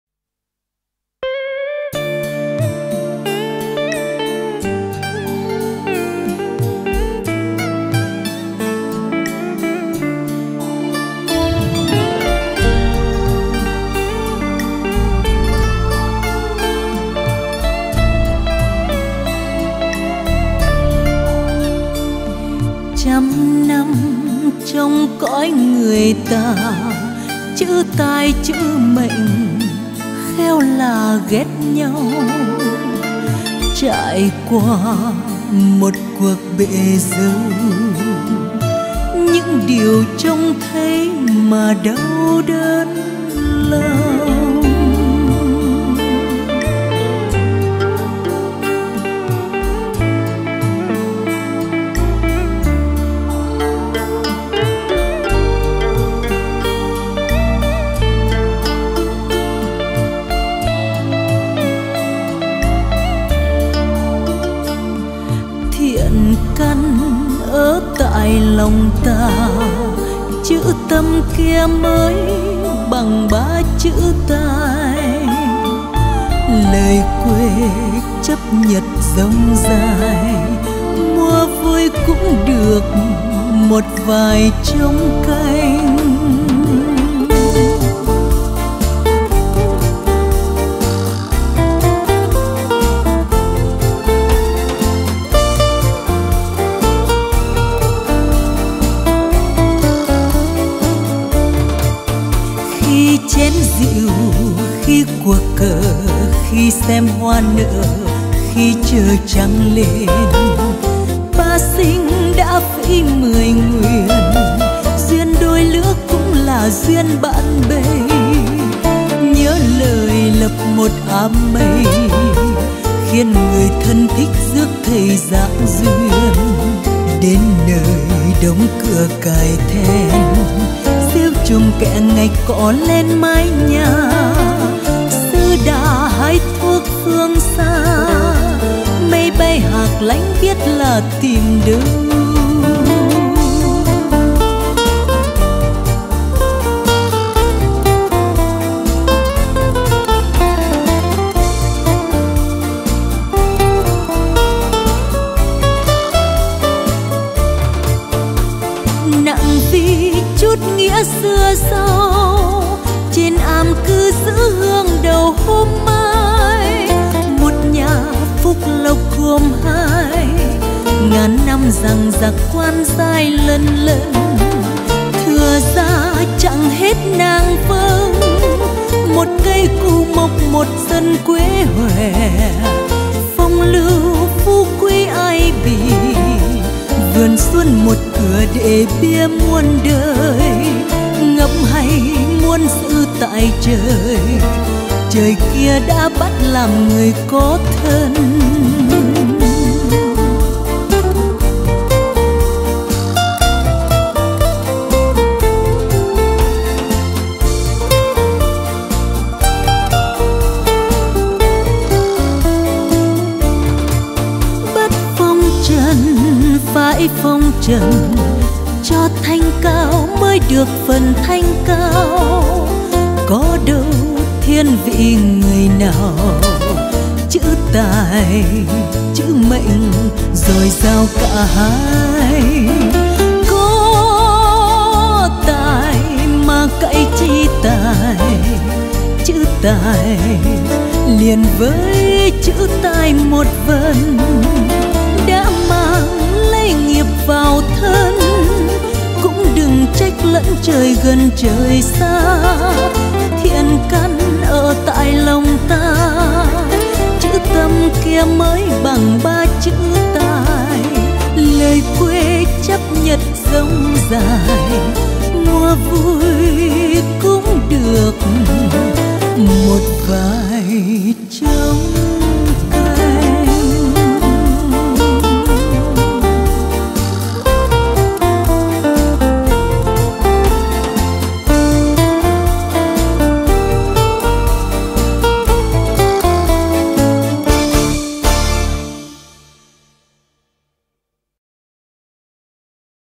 Kiều History 77 - Chữ Tài Chữ Mệnh - Talent et Destinée
CD-KVK7-11, de vers 3223 à vers 3254.
Traduction : Nguyen Van Vinh
Voix : Quynh Lan
Tantôt ils regardèrent s’ouvrir les fleurs tantôt ils jouirent ensemble du spectacle du lever de la lune.
Tous les vœux qu’ils avaient faits depuis longtemps étaient de la sorte exaucés.
La vie en commun des époux est après tout la vie en commun d’une paire d’amis.
Comme cela avait été convenu, ils firent bâtir un temple dans un coin bien isolé.
Et envoyèrent une personne de confiance auprès de la religieuse Giac Duyên pour la prier de venir.
Cette personne envoyée en arrivant sur les lieux trouva les portes fermées et les verrous tirés.
Les bas des murs était couvert de mousse, l’herbe montait jusqu’au toit de la maison.
La bonzesse était allée herboriser dans des régions lointaines.
Où chercher le nuage qui s’est envolé et l’oiseau qui s’est enfui ?
En souvenir de cette longue amitié.
Elle se voua à l’entretien du feu et de l’encens dans le temple.
Depuis lors, le bonheur et la richesse régnèrent dans la famille.
Les années s’écoulèrent heureuses : le mandarin monta peu à peu en grade.
Madame Vân avait assez à s’occuper avec le ménage.
L’arbre familial grandit et vieillit faisant de nombreux rejetons.
Elégance, aisance, richesse, honneurs, ils jouirent de tout cela comme personne n’en a jamais joui.
Et cette maison où la joie et le bonheur ont régné a laissé son souvenir aux dix mille générations à venir.
Elle est bonne à méditer cette grande vérité : tout vient du Ciel.
Le Ciel en nous condamnant à vivre en hommes, nous assigne à chacun un sort.
S’il nous a voués à une vie d’aventures, il nous faut accepter la vie d’aventures.
C’est seulement lorsqu’il a voulu nous faire honorable et grands que nous pouvons être honorables et grands.
Il ne se montre partial vis- à-vis de quiconque.
Talent, Destinée, ce sont deux grandes choses.
Quand vous êtes doué de talent, ne vous fiez pas trop à votre talent.
Tai talent ne rime-t-il pas avec Tai, malheur ?
Chacun de nous porte avec soi son karma.
Il ne faut jamais se plaindre que le Ciel est trop près ou trop loin.
Le bien a sa racine dans notre cœur.
Le « Cœur » à lui tout seul vaut trois fois plus que le « Talent ».
Ces propos vulgaires je les ai pris un peu partout par ci par là pour en faire ce recueil sans prétention.
Ils vous procureront toujours quelque distraction pendant quelques veilles.